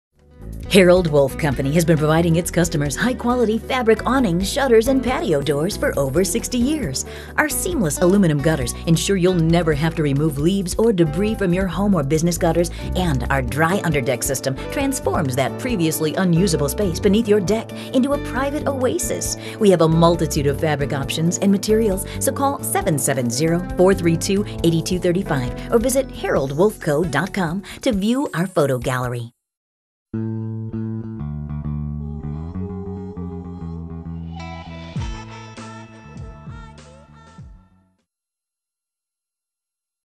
Harold Wolfe Company
Harold Wolfe Company, in Atlanta, Georgia, specializes in design and installation of custom fabric awnings, gutters and patio doors to give your home that extra something. Their gutters also protect your windows and awnings so they will last for years to come.